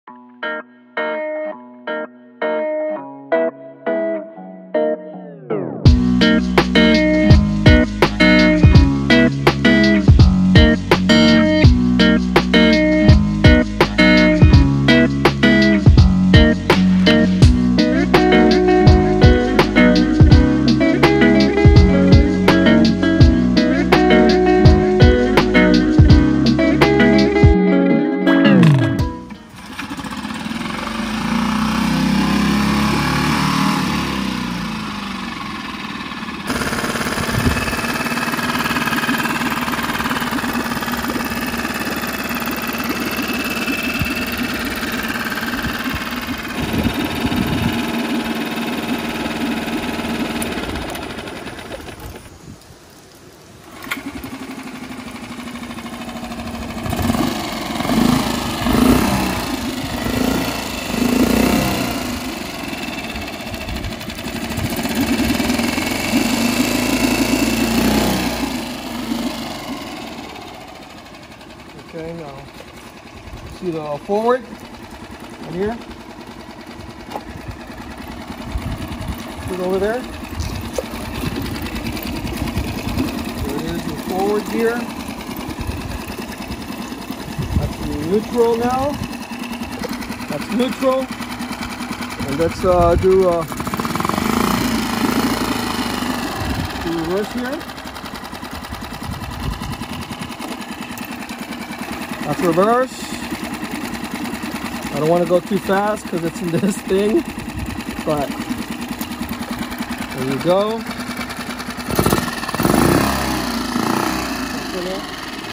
Solo Skiff with 6hp Tohatsu for sale!
Up for sale is my 2017 Solo Skiff with a 6hp Tohatsu outboard and trailer. There are way too many extras to list, but you'll be happy to know you can pretty much just go out and fish your favorite spot immediately.